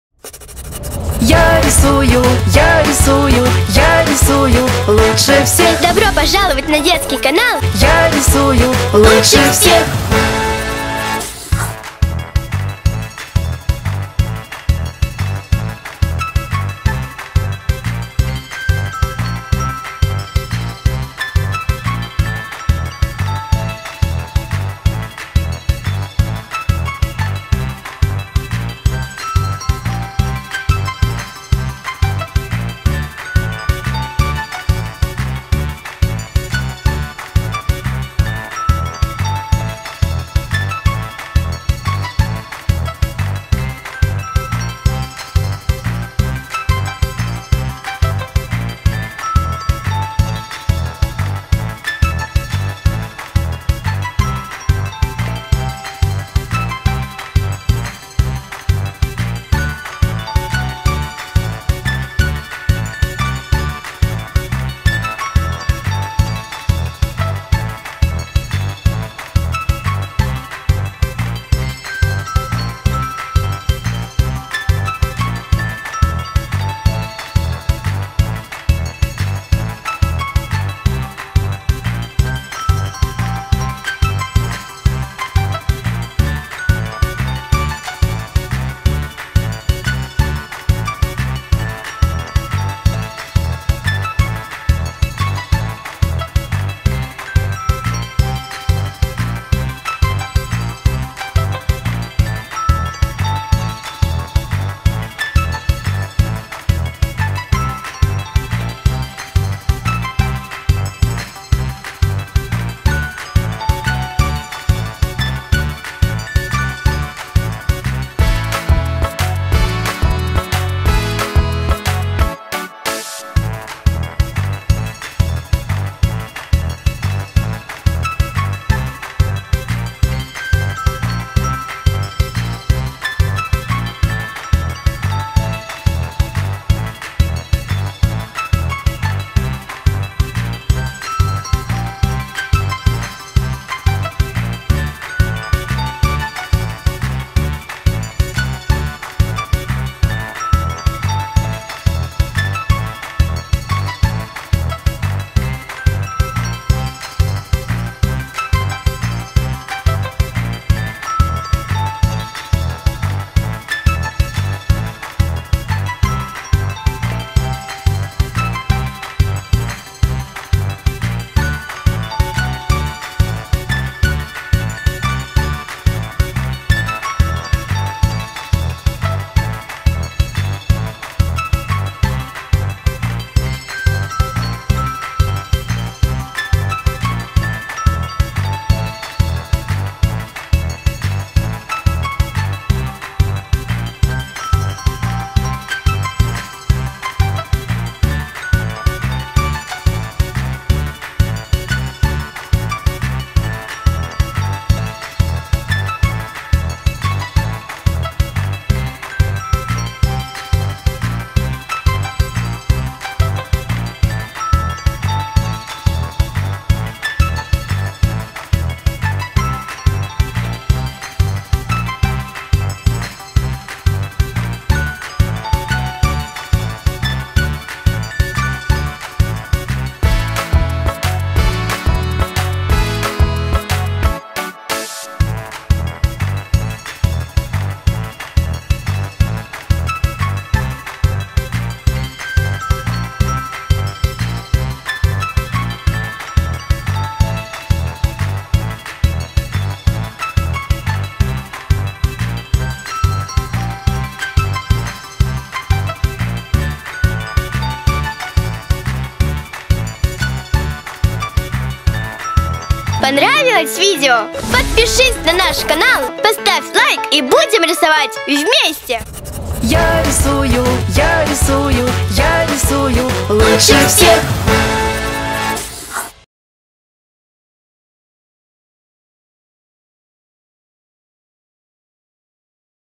Уроки Рисования Для Детей
Бесплатные уроки рисования для детей и родителей
Сегодня у нас МАЯК!
Нравится?)
Новые рисунки - каждый день)
Free drawing lessons for children
Today we have a LIGHTHOUSE!